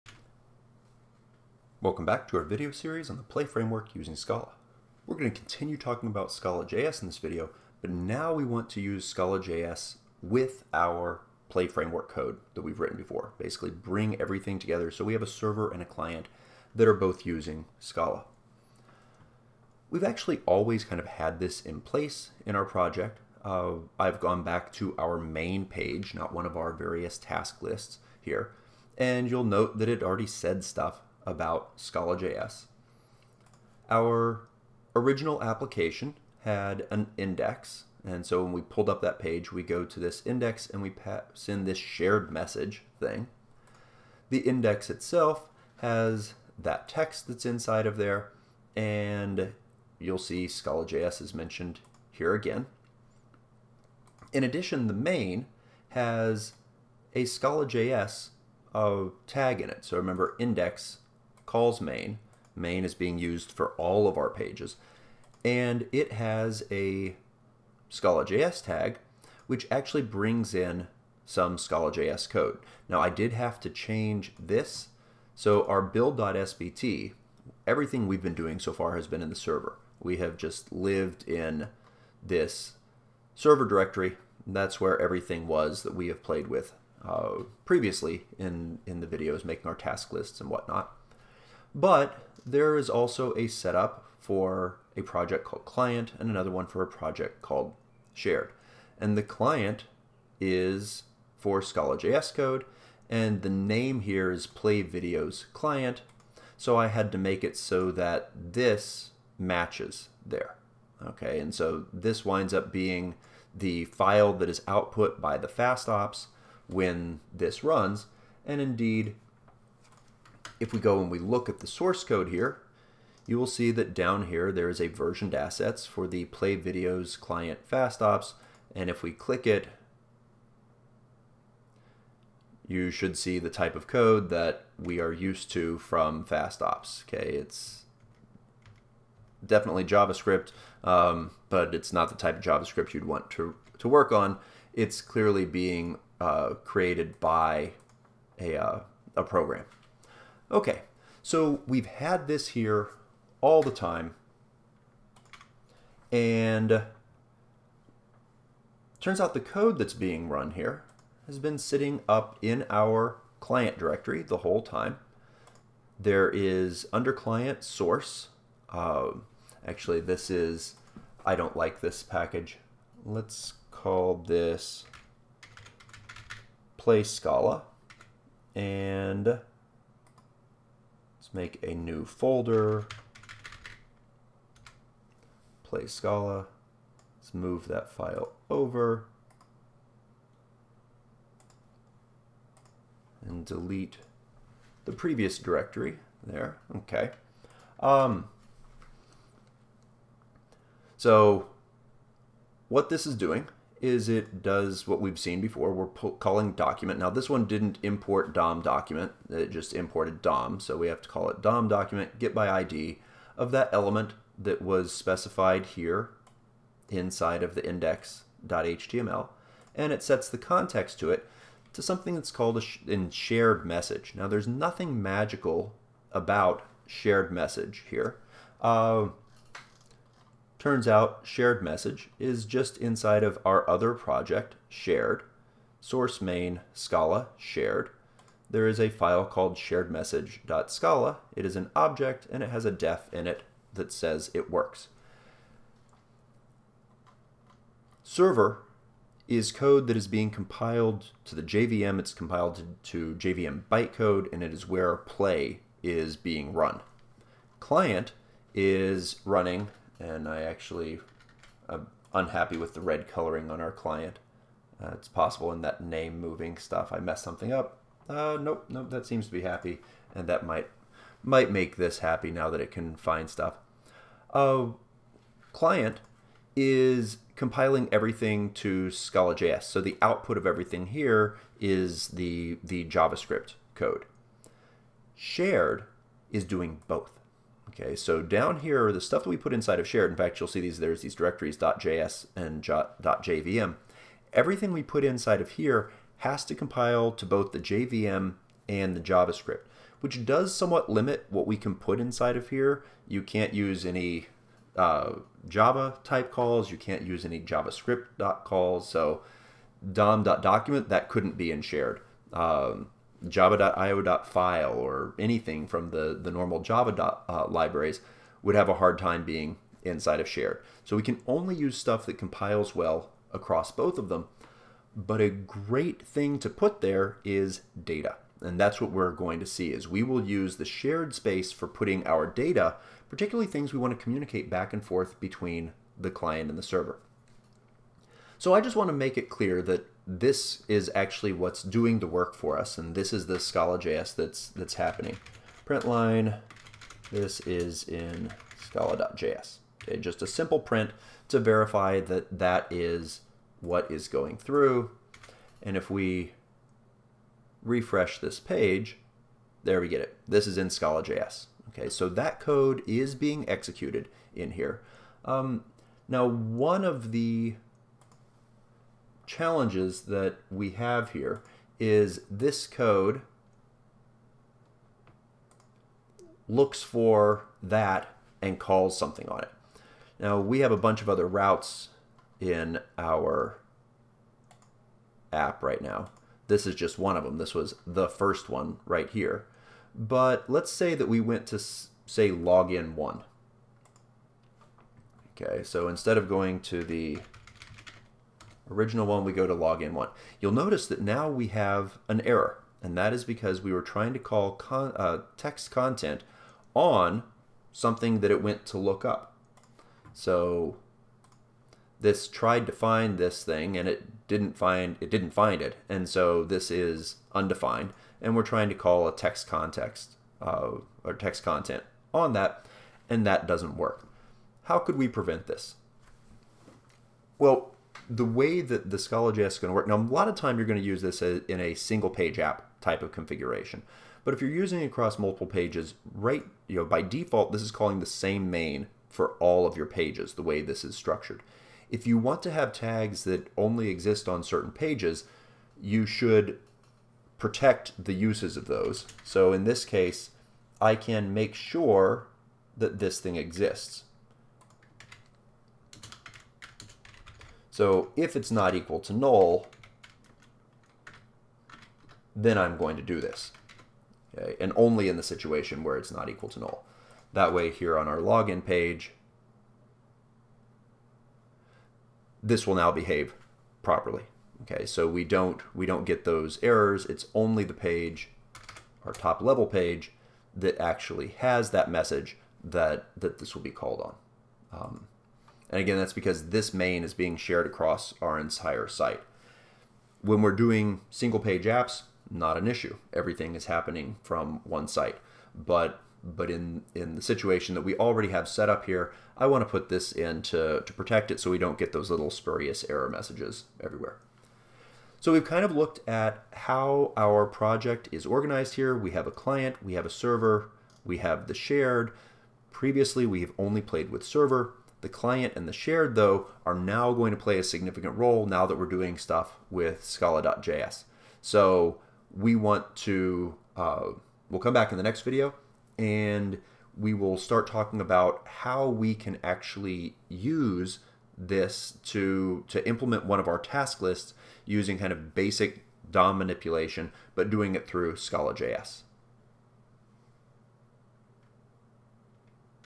Play with Scala.js Part 1 (Play using Scala)
This video looks at how Scala.js is set up in your larger Play projects.